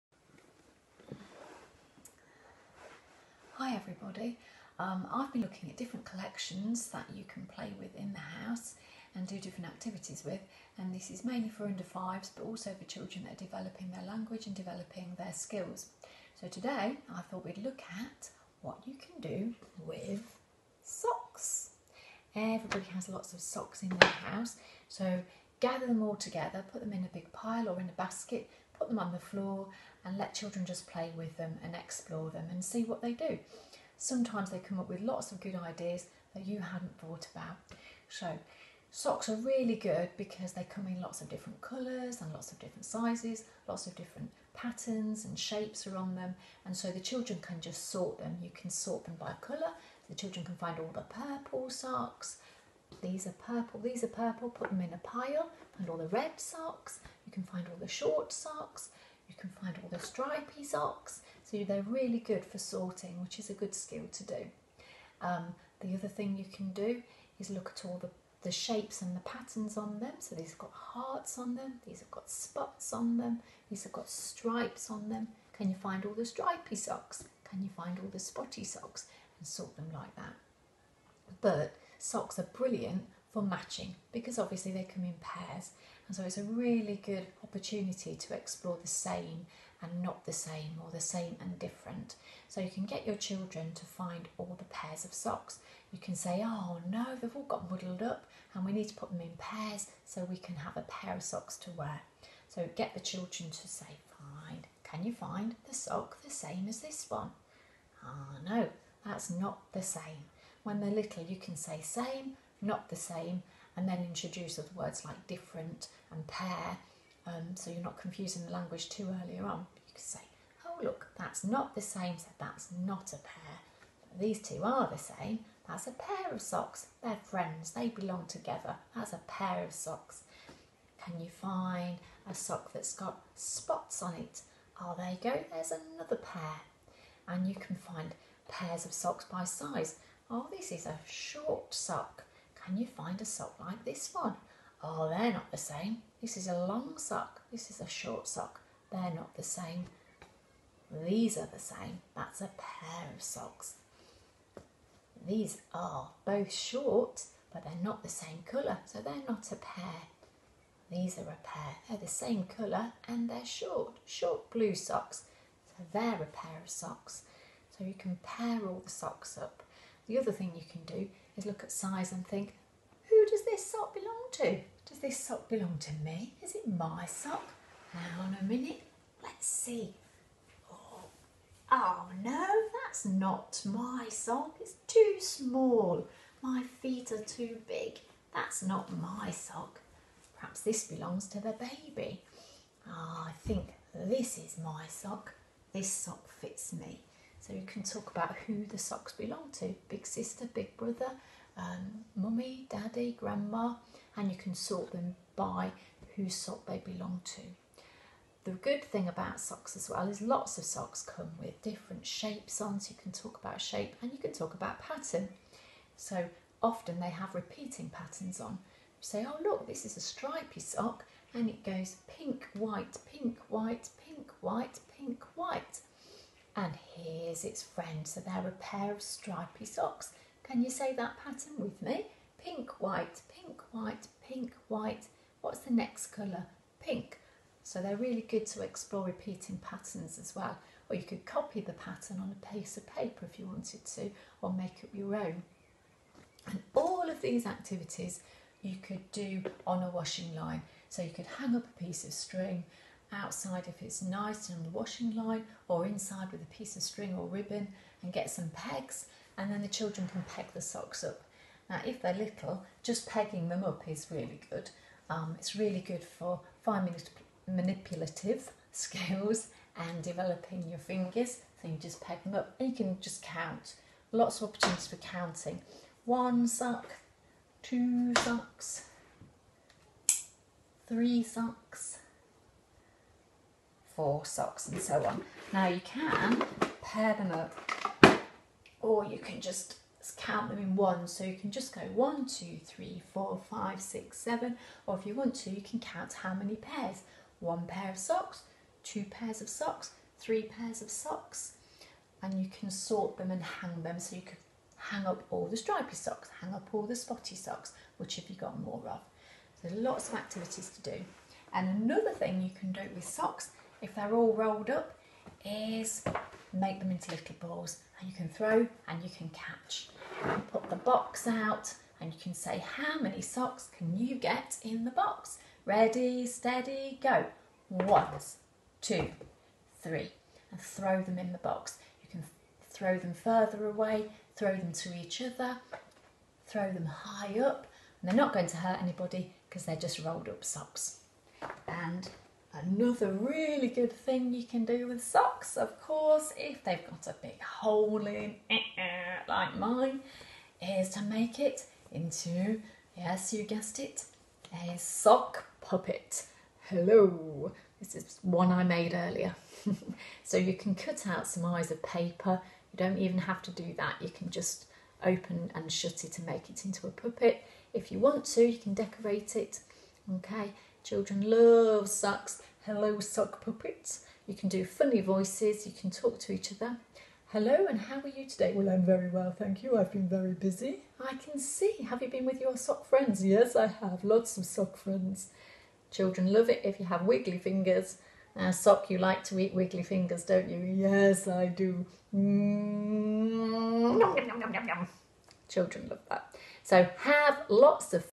Explore and learn: what to do with socks?
A great video to encourage children to develop language and communication through play using socks you have around the house.